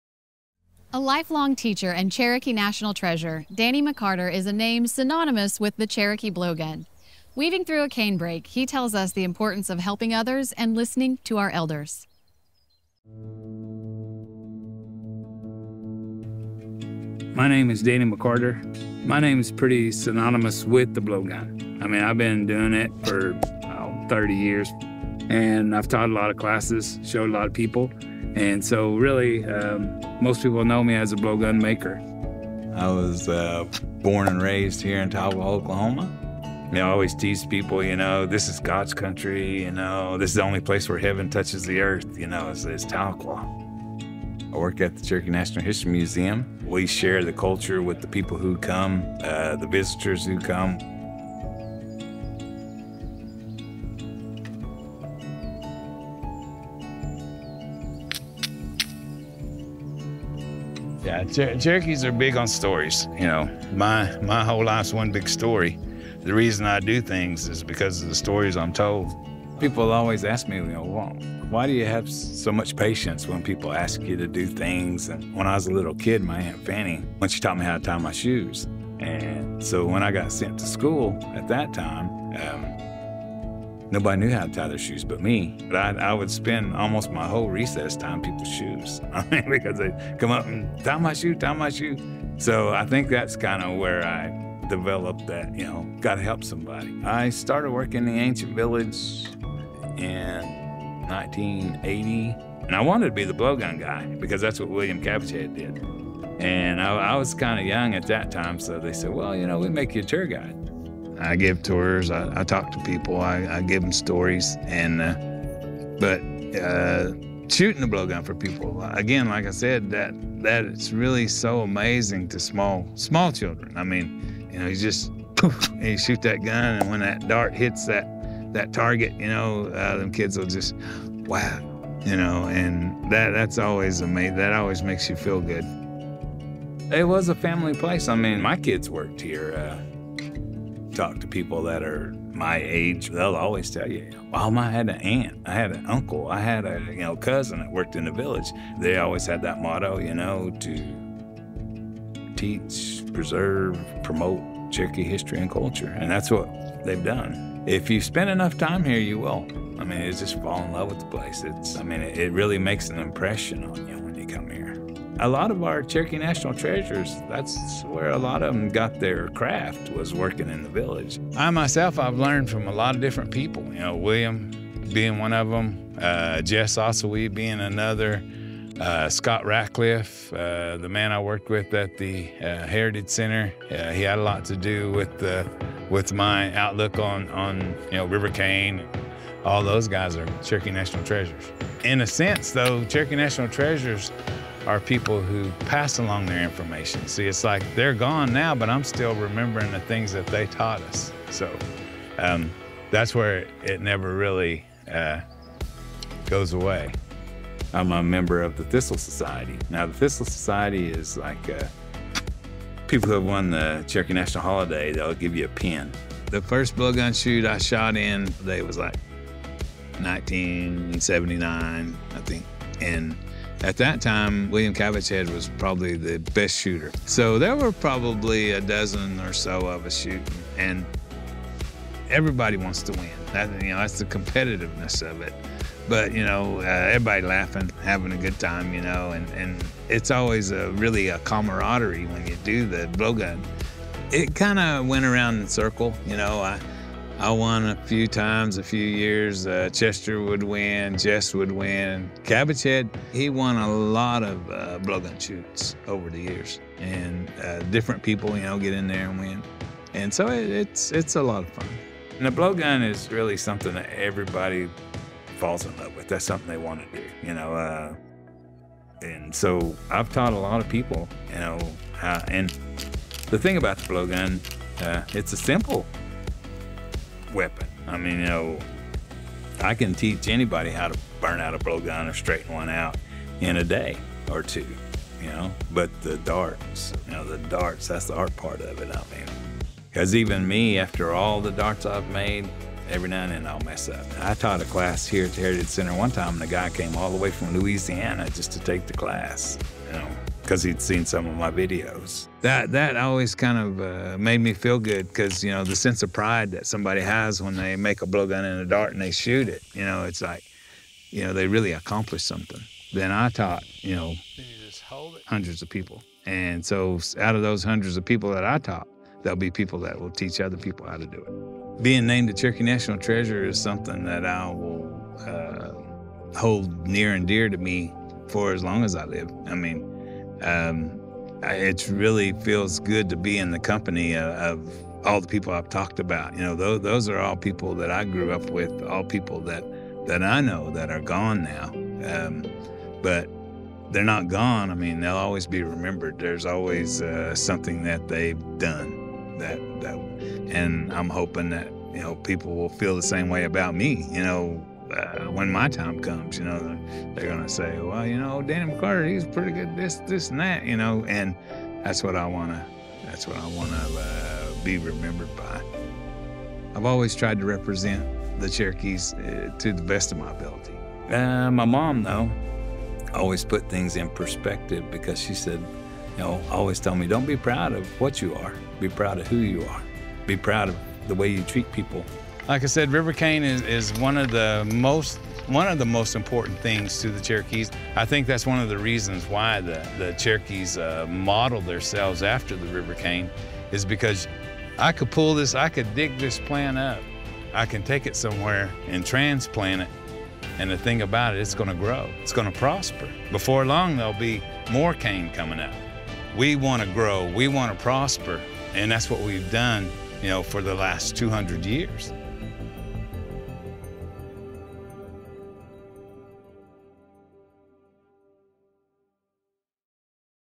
Teaching His Treasure: Blowgun Maker Danny McCarter
Few know the Cherokee Blowgun as well as Danny McCarter. As a lifelong teacher and Cherokee National Treasure, Danny shares the importance of helping others and listening to our elders.